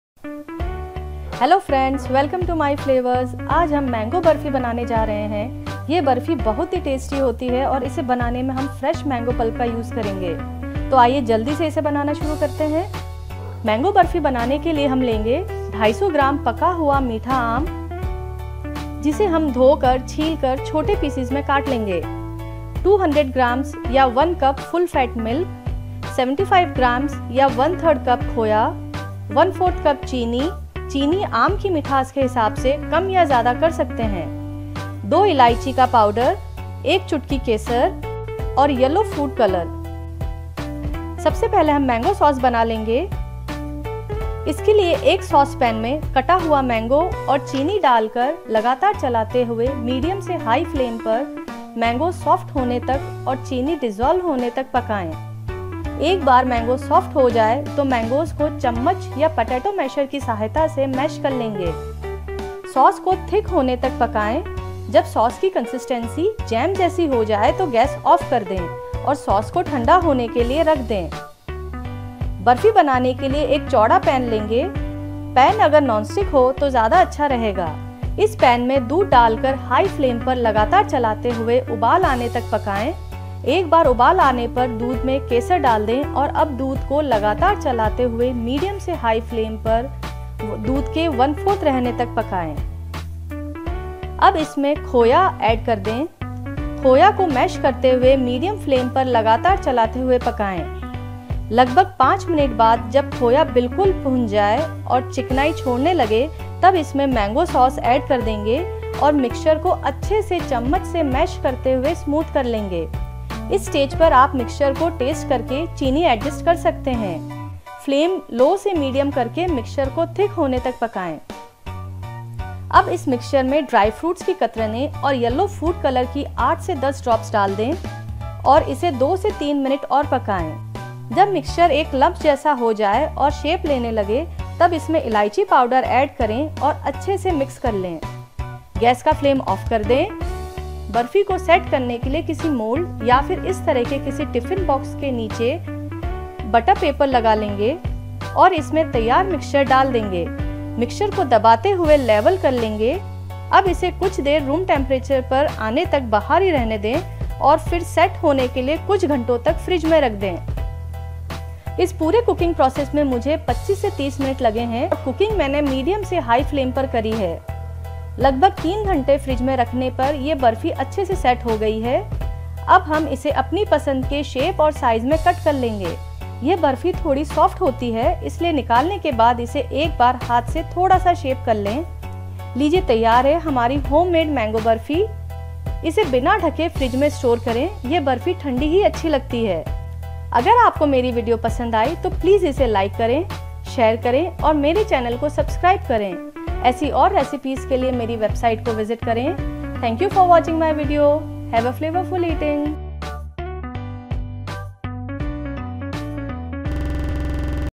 मिन्टों में बनाऐं स्वादिष्ट आम बर्फी/आम कलाकंद Easy Mango Burfi Recipe/Mango Khoya burfi/burfi
Today we are going to make Mango Burfi or mango kalakand. It is a very tasty bufri prepared using fresh mango pulp.

Ingredients:
• Mango - 250 gms
• Milk (full fat milk) - 200 lts / 1 cup
• Khoya – 75 gms / ½ cup
• Cardamom – 2 powdered
• Yellow food colour – 8-10 drops (optional)
• Dry fruit chopped (optional)